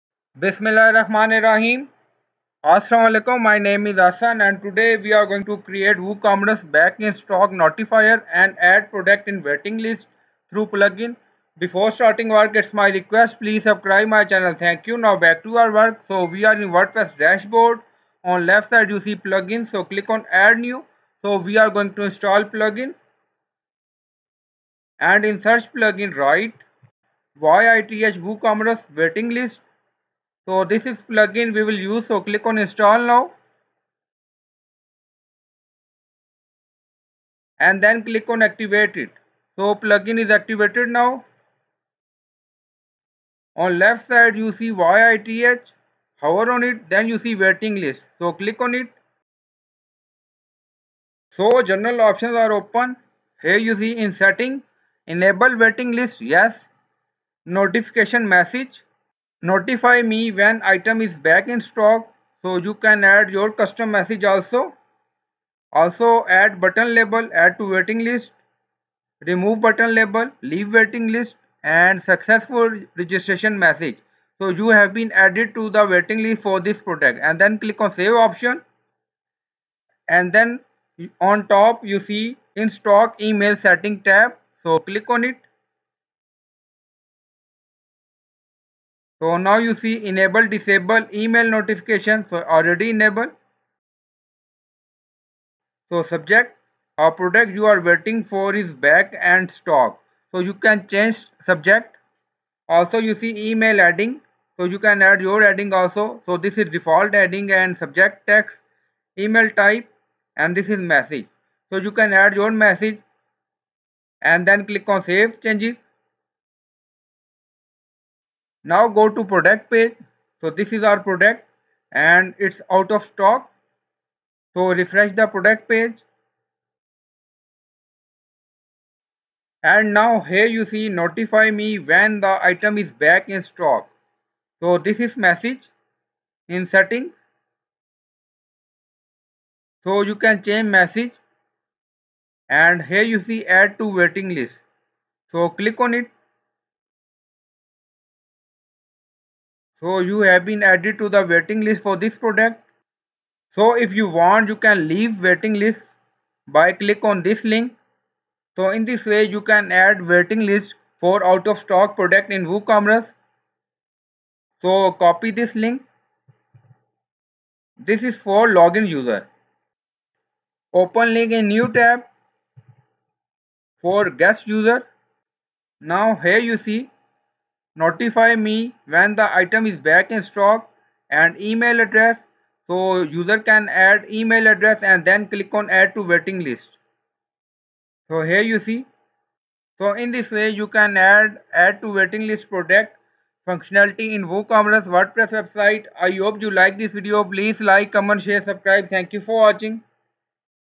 How to Create Woocommerce Back in Stock Notify and Waitlist | Add Product to Waiting List WordPress
In this wordpress tutorial for beginners you will learn how to add products in add to waiting list in woocommerce website using plugin. So when product is out of stock then user can add product in waiting list and when product will available then notify customer that item is back in stock.